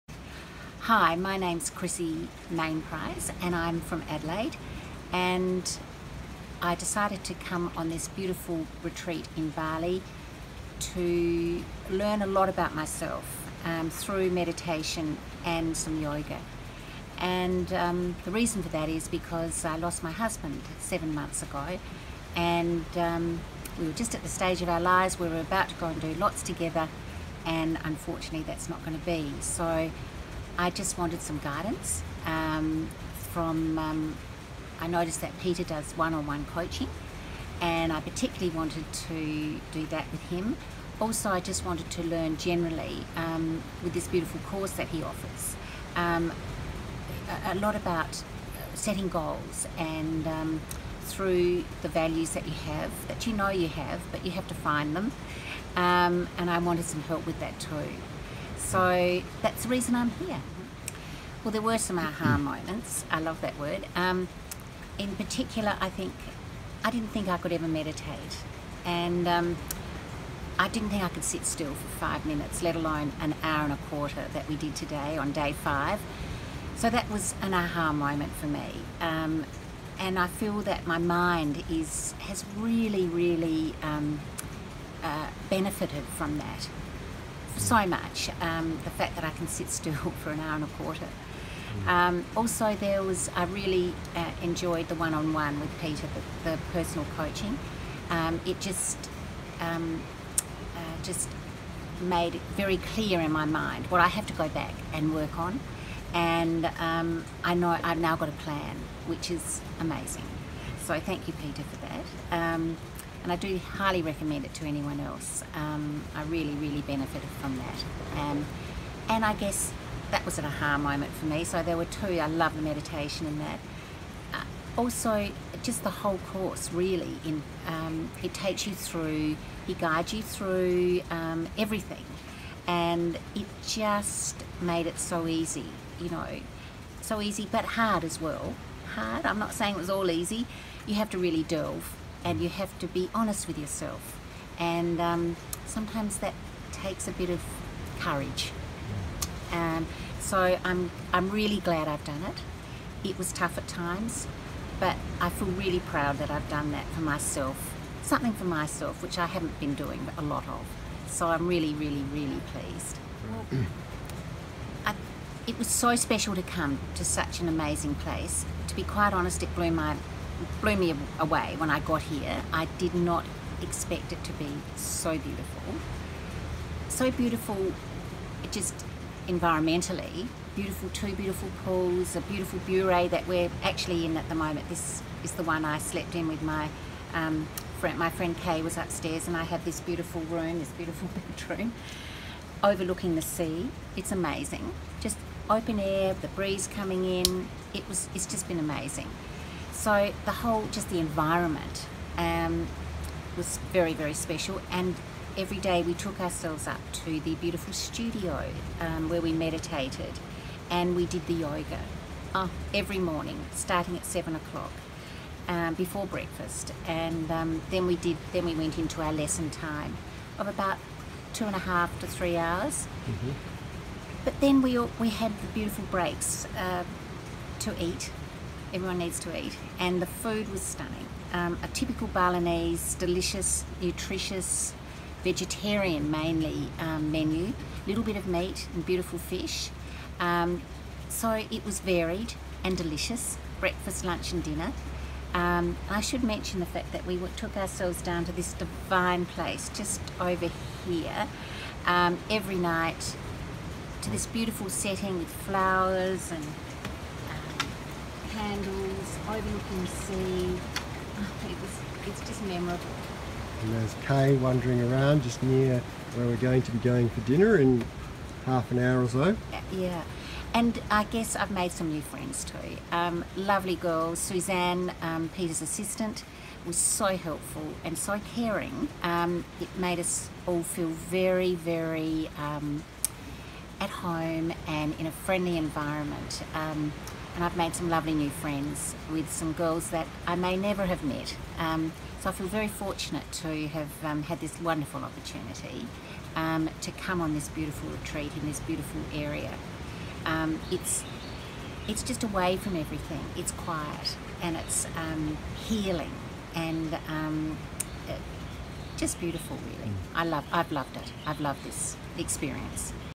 Chrissie Mainprize talks about Inner Growth Bali 18
Chrissie has had a difficult year - and so this retreat has set her back on a path to looking forward to the future